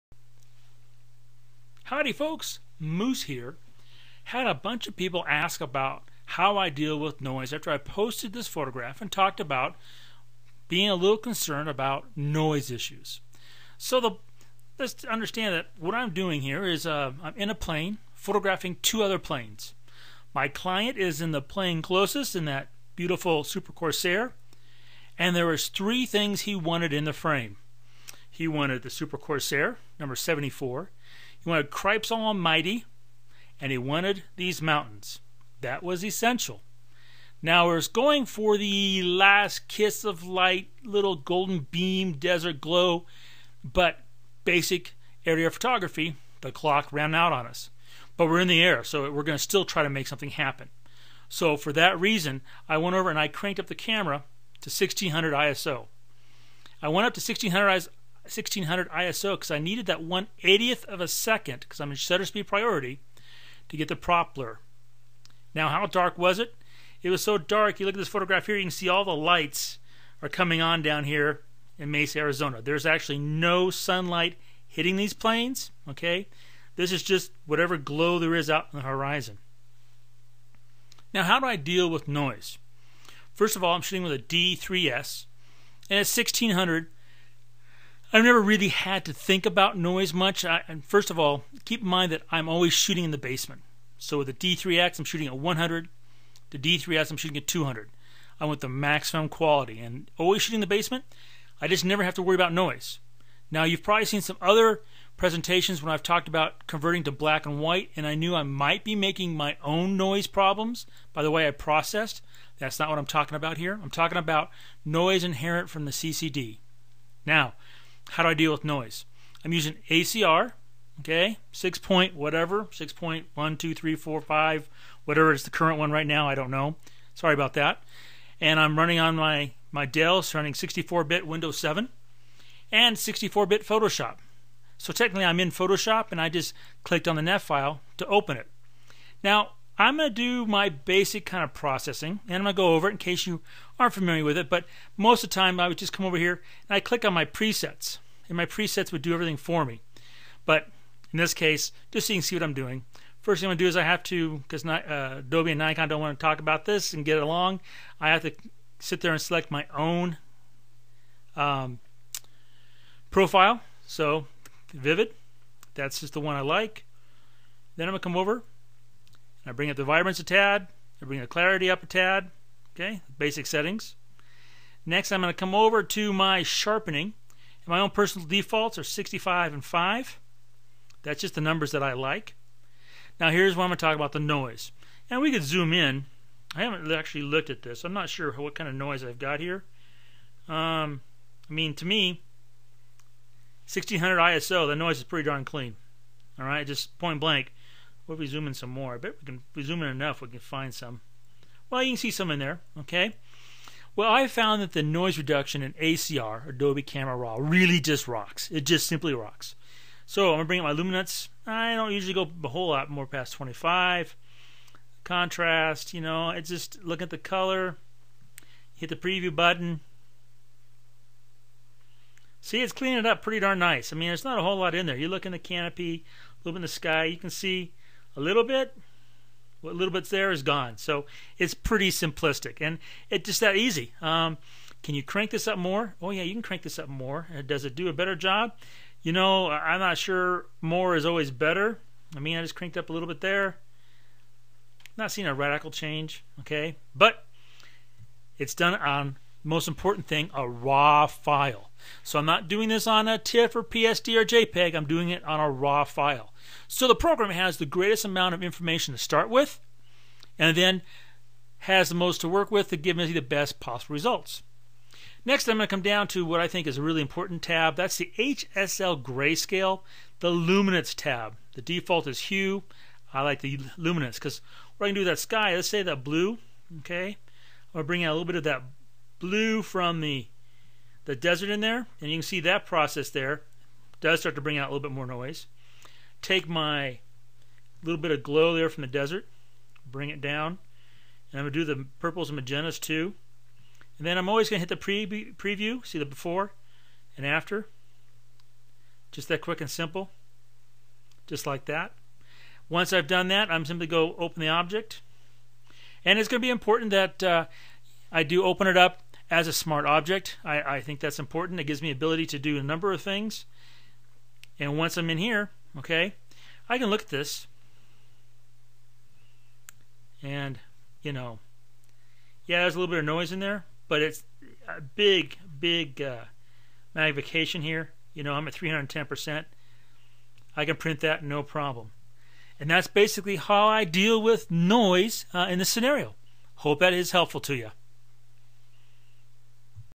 ACR & Noise, How to Deal with It
Here's how with ACR you can deal with Noise in a RAW file, the place to deal with it.